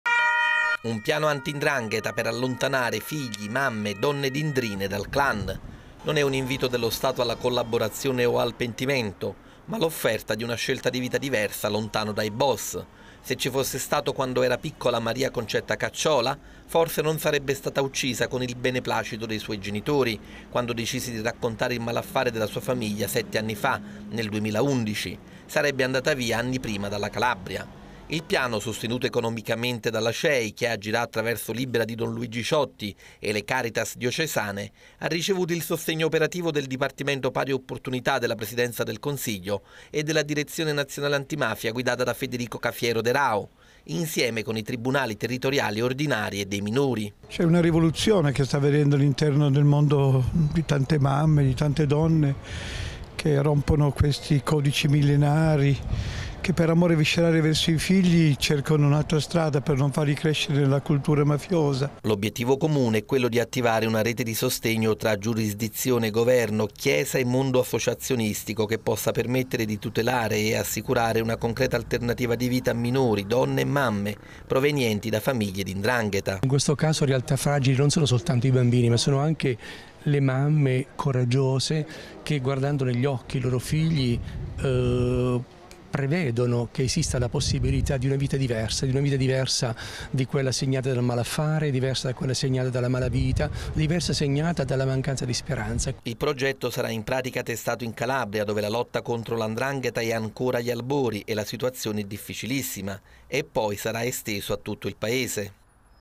Piano anti-ndrangheta per allontanare mogli e figli dai clan. Le voci di don Ciotti e Mons Galantino
Per tutelare e assicurare una alternativa di vita ai minori provenienti dalle famiglie legate alla criminalità organizzata, oggi, è stato firmato un protocollo d’intesa tra il governo, la Conferenza episcopale italiana e l’associazione Libera. Una rete di sostegno per tutelare e assicurare futuro ai figli e alle mogli dei boss che decidono di tagliare con le famiglie mafiose. Servizio di Luigi Ferraiuolo